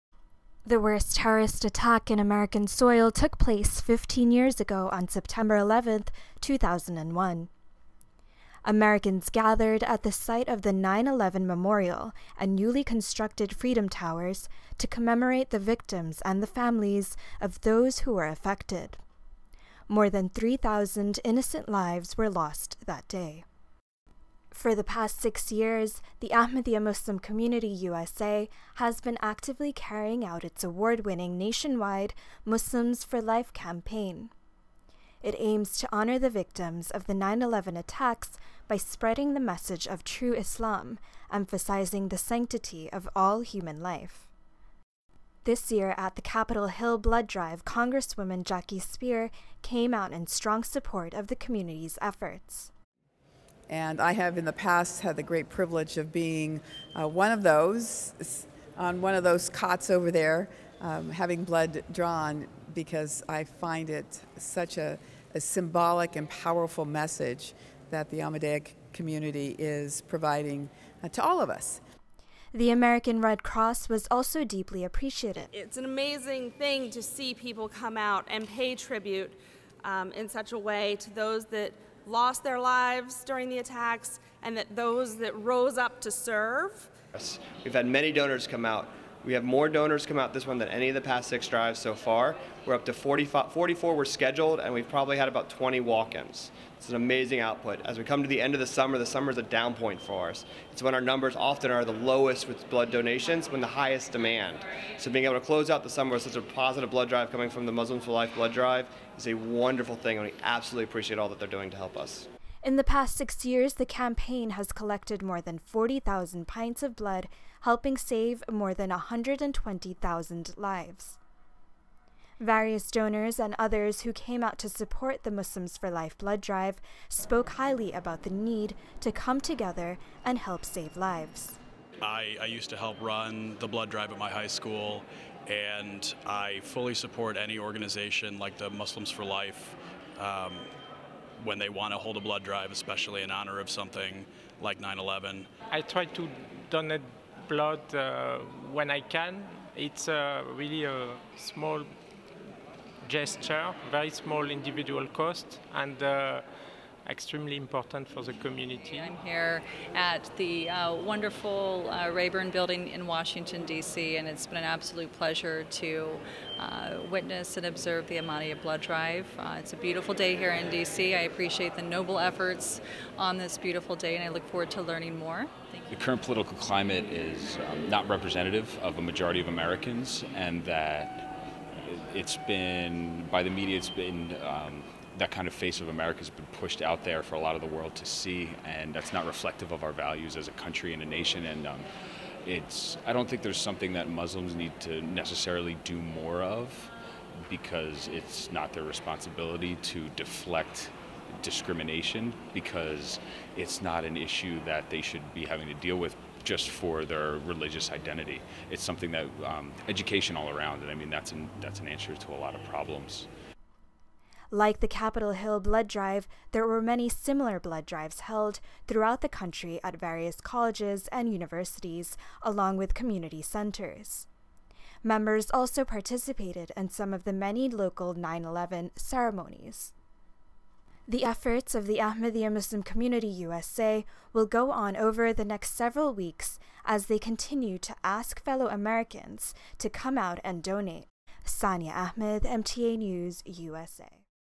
Muslims For Life - Capitol Hill 2016
News Clip about the Ahmadiyya Muslim Community's 6th annual Muslims For Life - 9/11 Blood Drive at the U.S. Capitol in Washington D.C.  The event took place on 9/9/16 in the Rayburn Office Building Foyer.